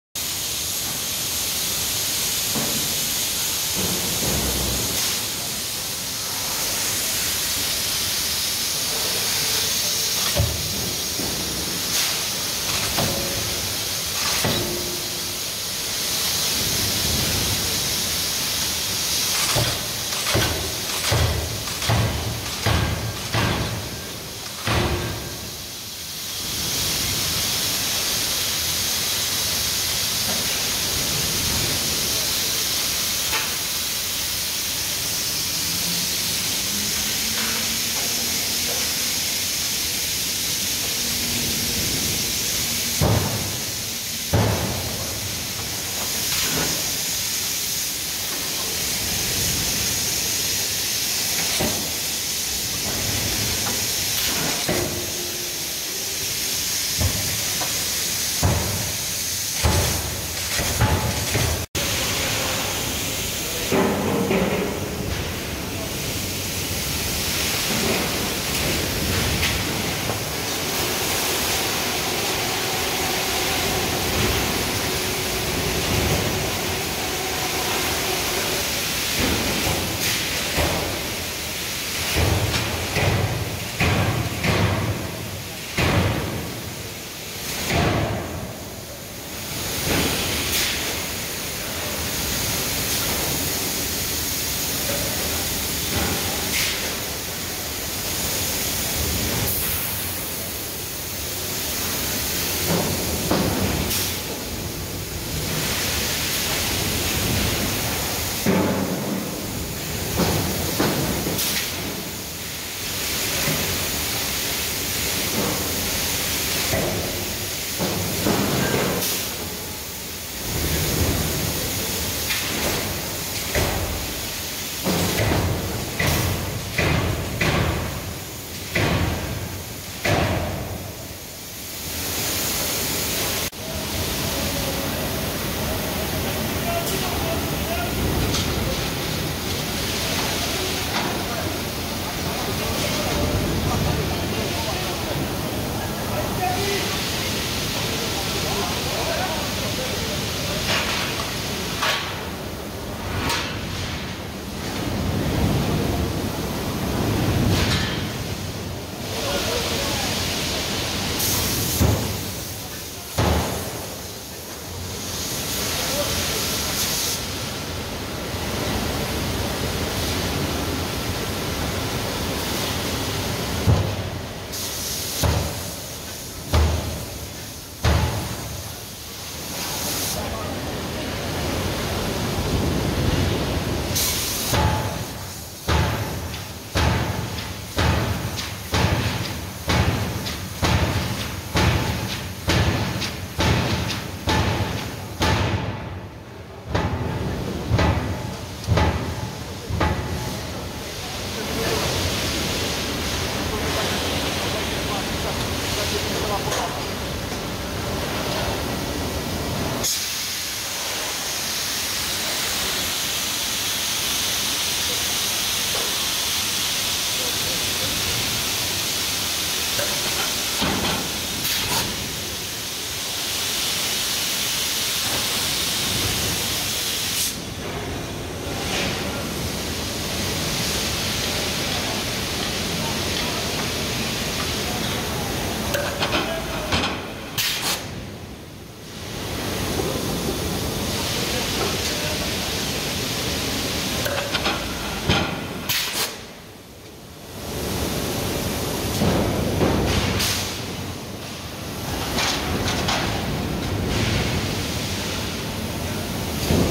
Kovárna MSV Metal Studénka
Kování dílů pro spřáhla a nárazníky kolejových vozidel